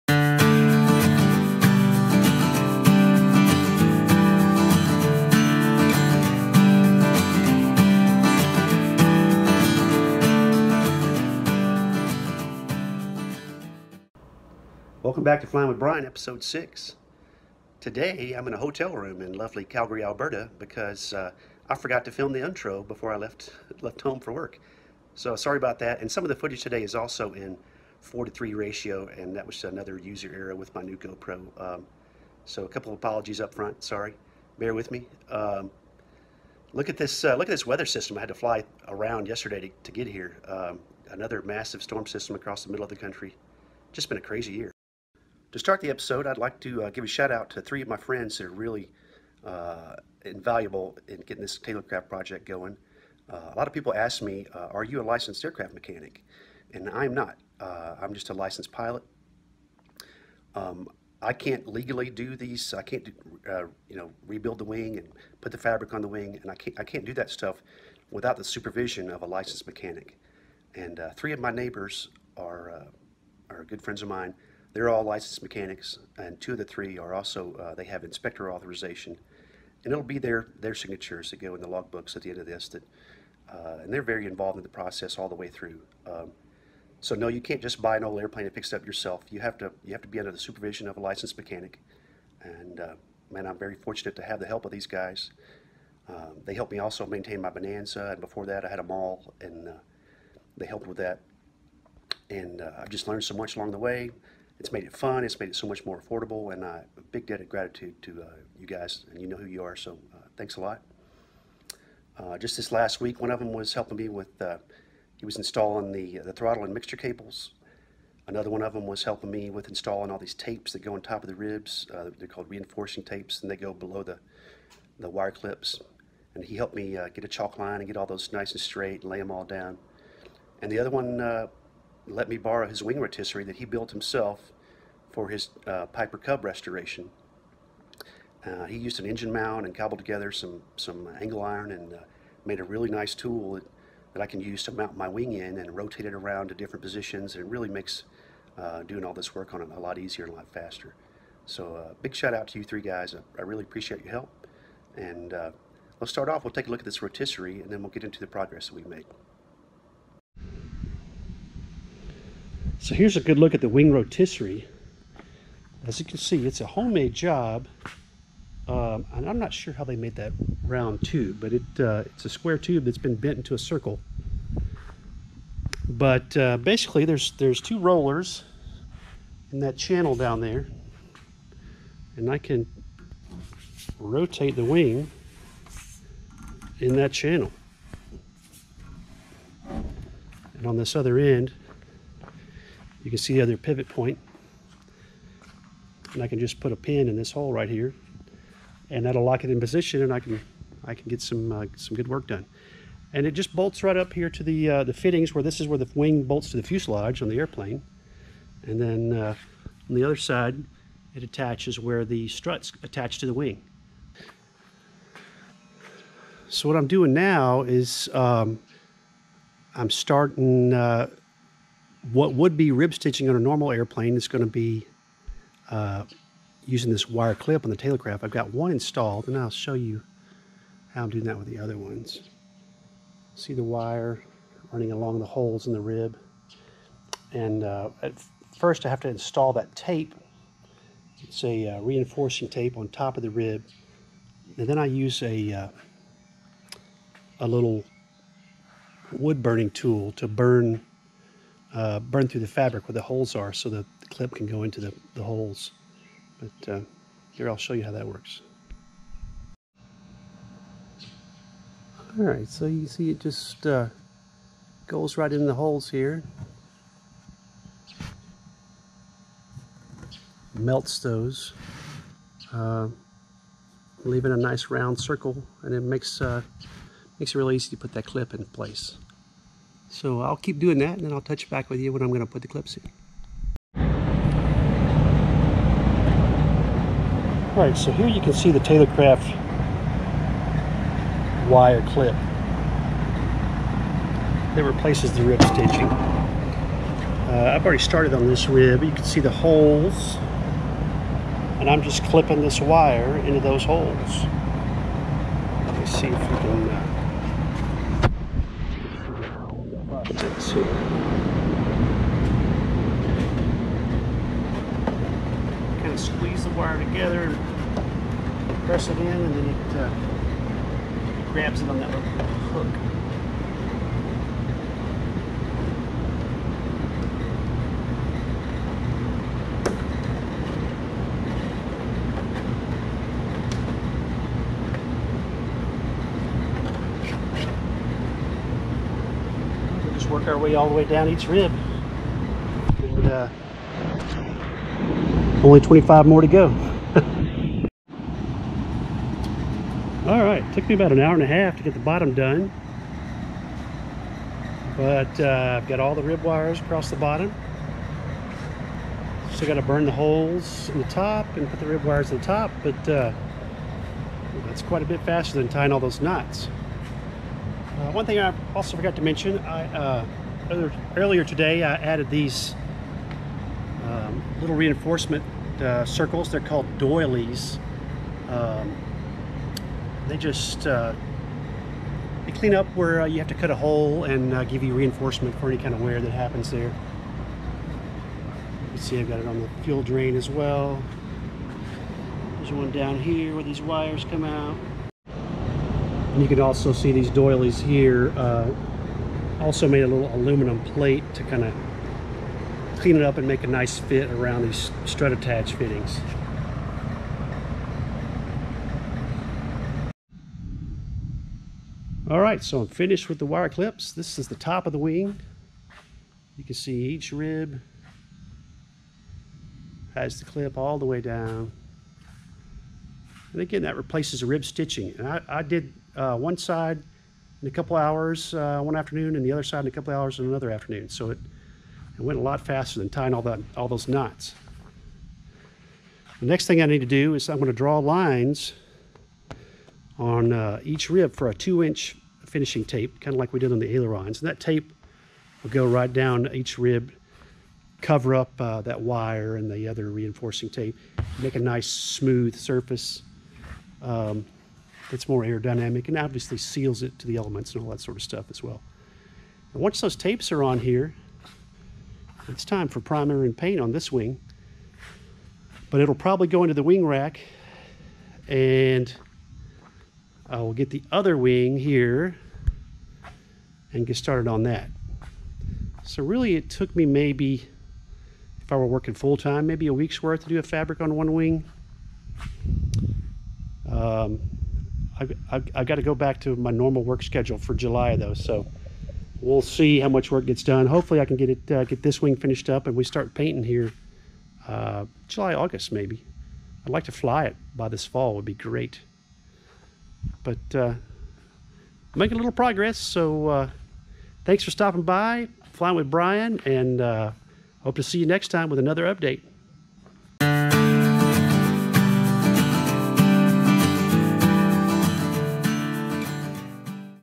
Episode 06, Installing Taylorcraft wire clips to wing fabric
A look at a wing rotisserie tool, and an update on the left wing fabric covering process - including the installation of Taylorcraft wire (used instead of rib stitching).